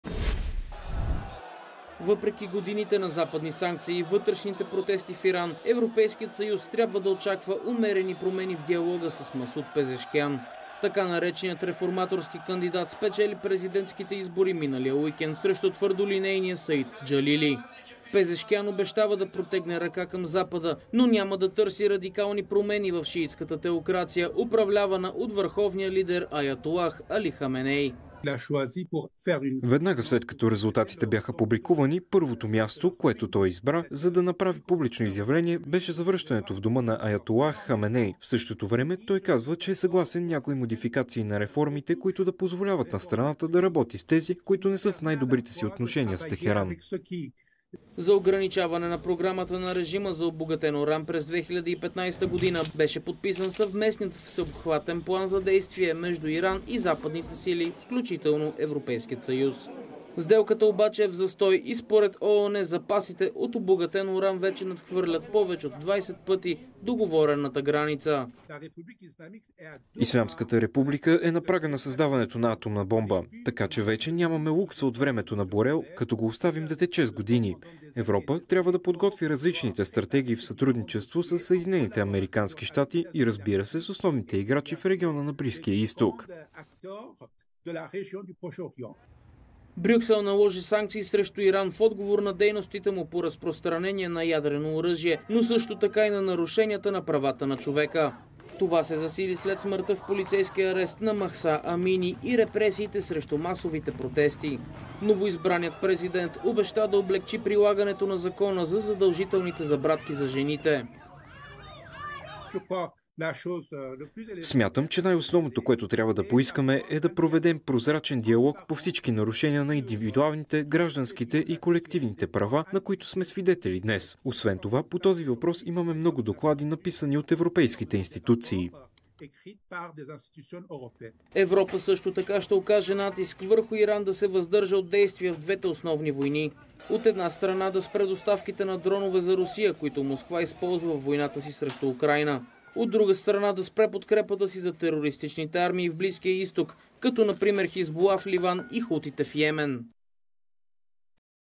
Как ще се отрази победата на реформатора Масуд Песешкиан на отношения отношенията ЕС – Иран?
След победата на Масуд Пезешкян на президентските избори в Иран, предстоят важни промени в страната. Един от основните въпроси обаче е как ще се отрази новата власт върху отношенията между Брюксел и Техеран. Разказват колегите ни от Euronews Франция.